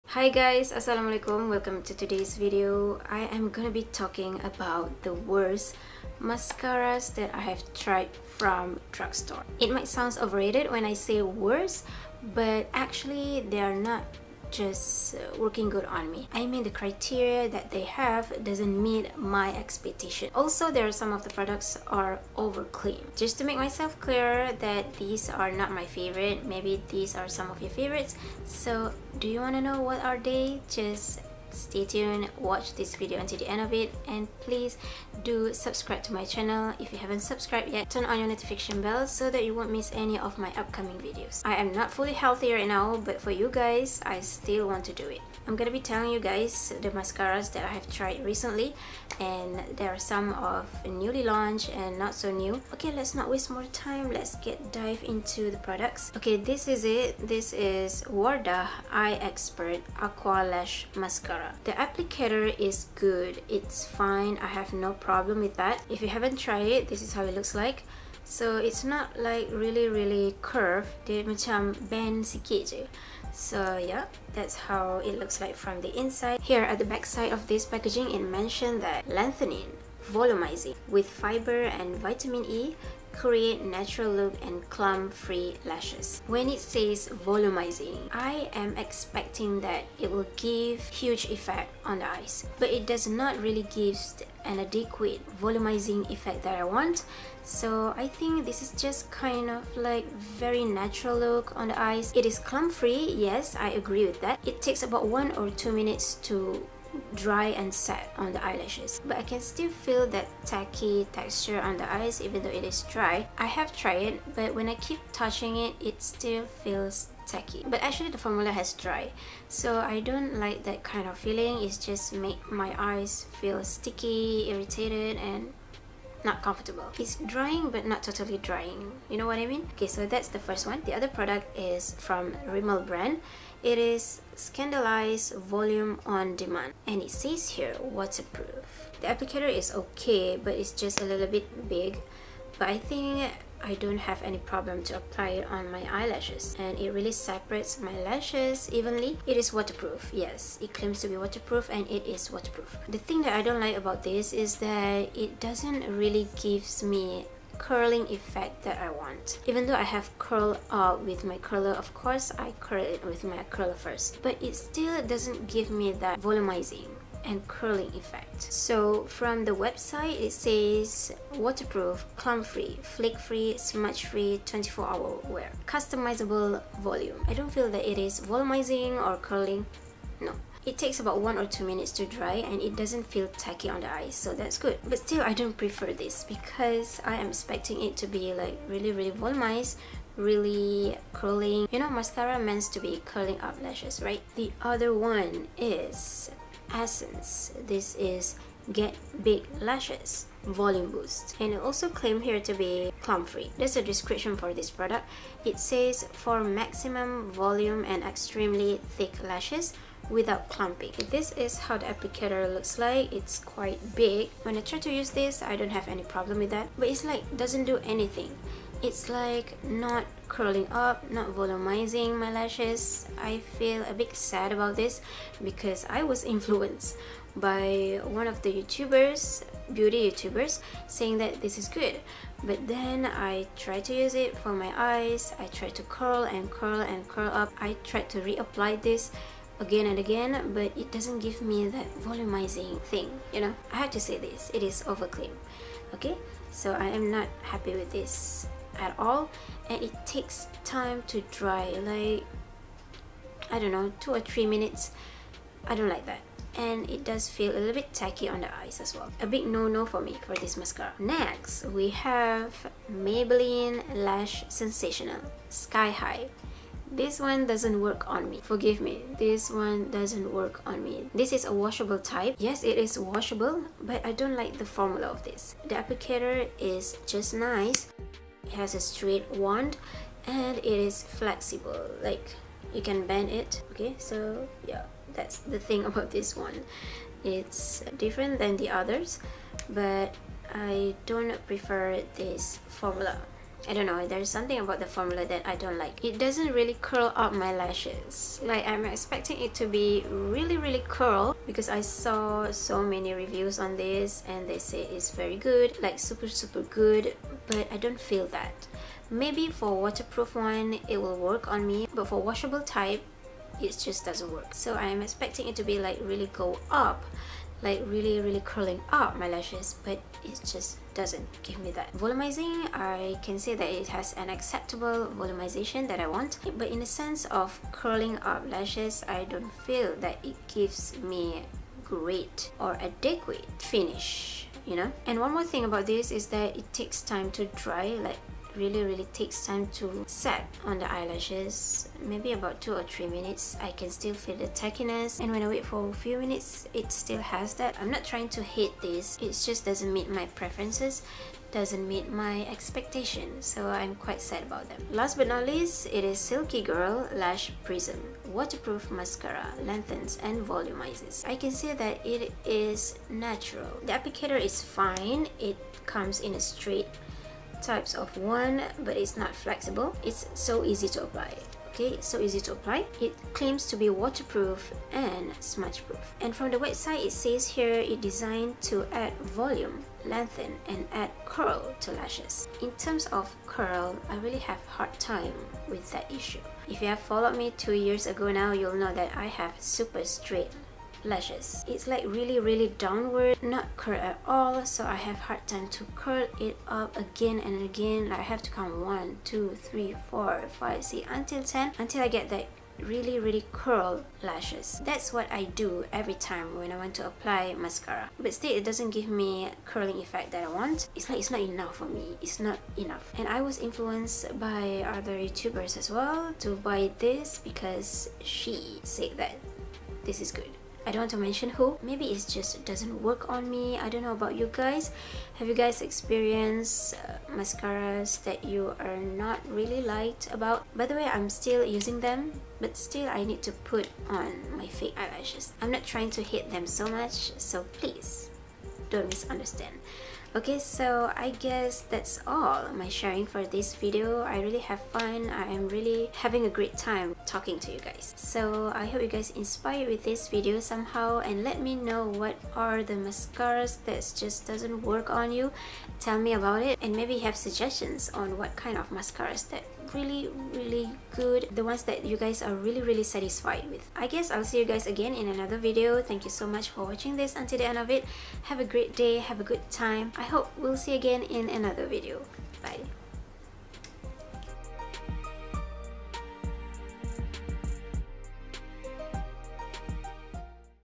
Drugstore FAILED MASCARA 2022
Forgive me that these are just not the best for me.Tell me your opinions about them if you had one.

P R O D U C T S  R E V I E W E D

Wardah Eye Expert Lash mascara

Rimmel Scandaleyes mascara

Essence Volume Boost mascara

Maybelline Sky High mascara

Silkygirl mascara

M U S T -  W A T C H